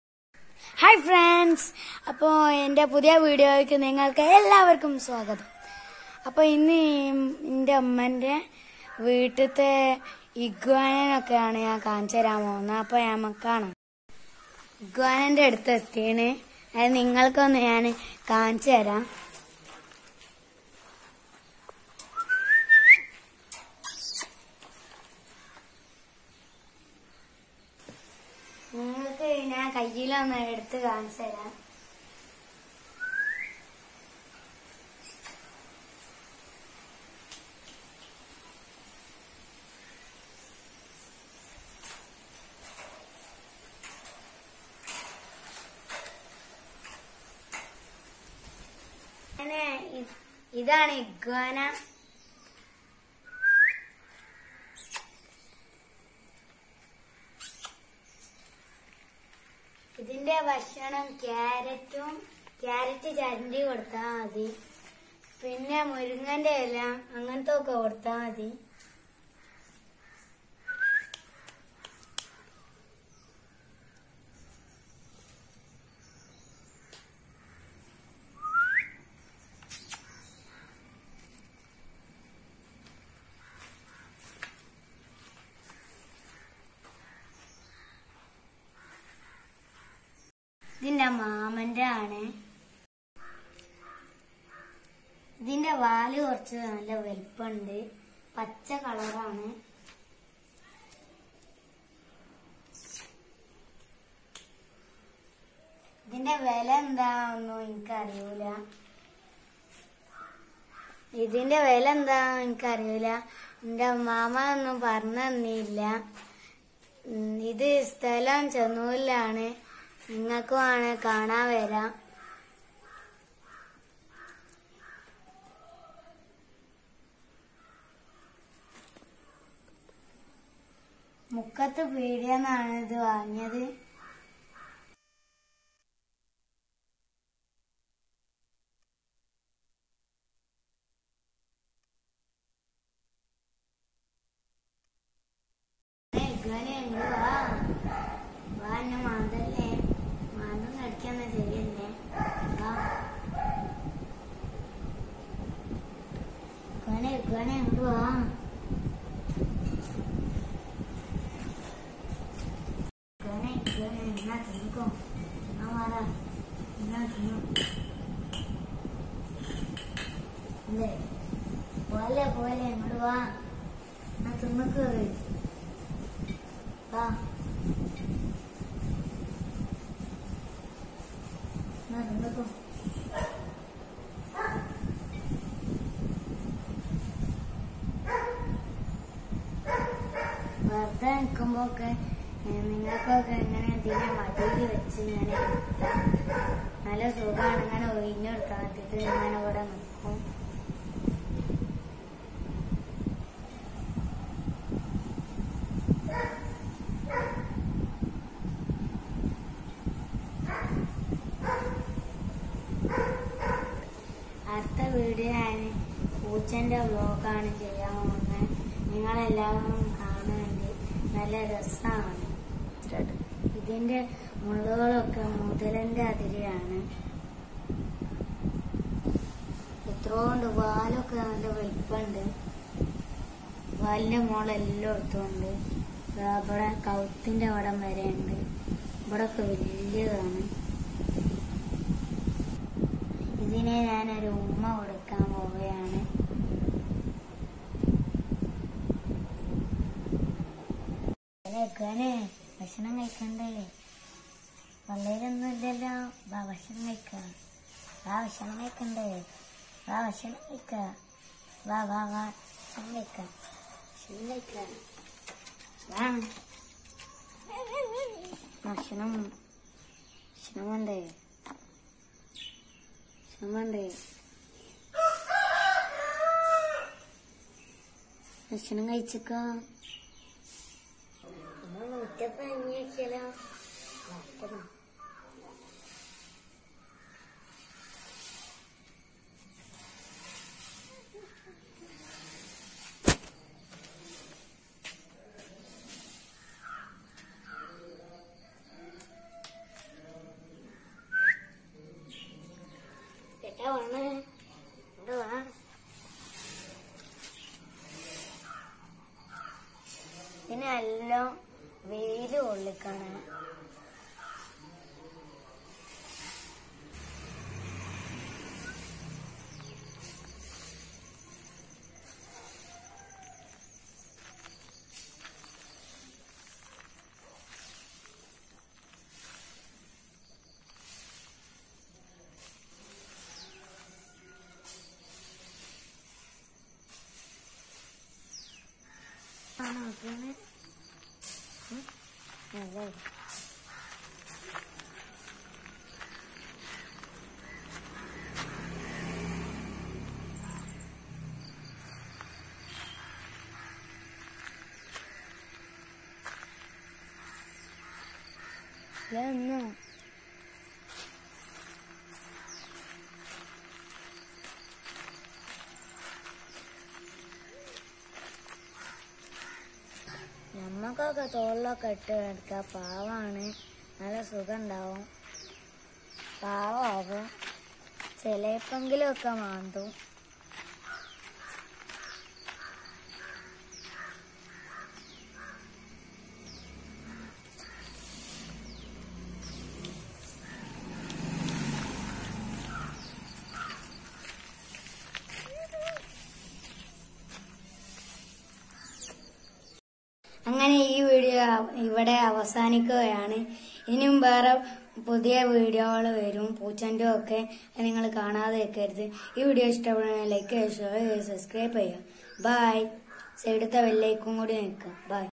IGUANA PET 🦎| ഇഗുആന കടിക്കുമോ?😳 | malayalam | Home Pets
Iguana (/ɪˈɡwɑːnə/,[1][2] Spanish: [iˈɣwana]) is a genus of herbivorous lizards that are native to tropical areas of Mexico, Central America, South America, and the Caribbean. The genus was first described in 1768 by Austrian naturalist Josephus Nicolaus Laurenti in his book Specimen Medicum, Exhibens Synopsin Reptilium Emendatam cum Experimentis circa Venena. There were formerly considered to be only two species in the genus; the green iguana, which is widespread throughout its range and a popular pet, and the Lesser Antillean iguana, which is native to the Lesser Antilles; however, genetic analysis indicates that the green iguana may comprise a species complex of multiple species, some of which have been recently described.[3]